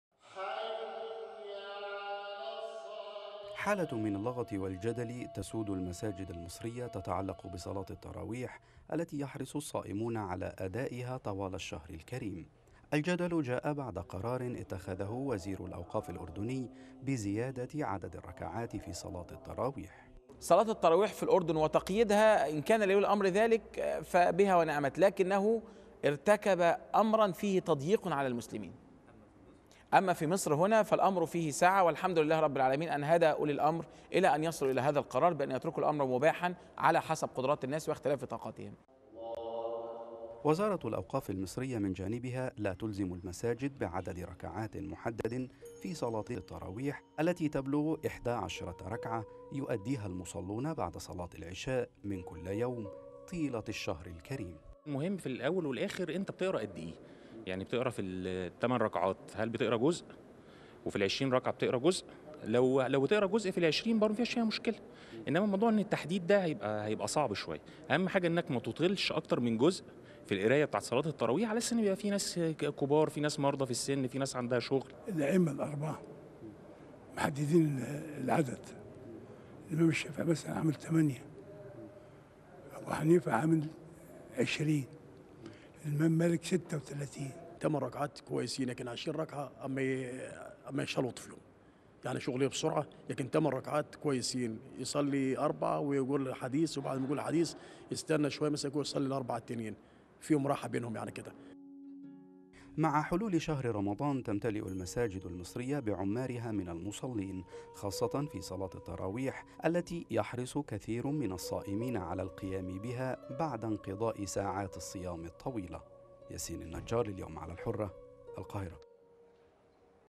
تراويح رمضان.. سجال يتجدد كل سنة
كما كل عام تجدد السجال السنوي عن عدد ركعات صلاة التراويح